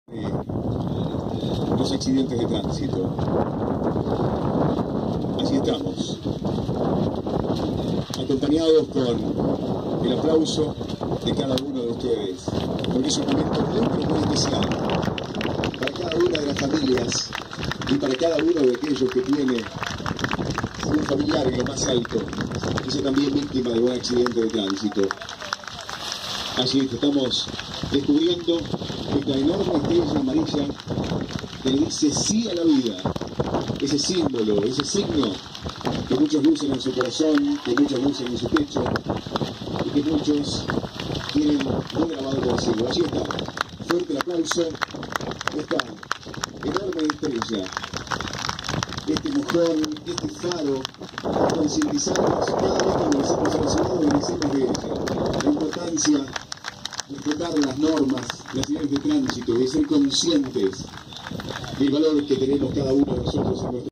El recuerdo y homenaje a 10 años de la tragedia de la combi
La asociación Estrellas Amarillas, descubrió un monolito como un punto más para reforzar la concientización sobre la educación vial y en memoria de 4 jóvenes que perdieron la vida en aquella tragedia. El monolito se emplaza en en la avenida Pringles entre Elpidio González y Calle Nº 108.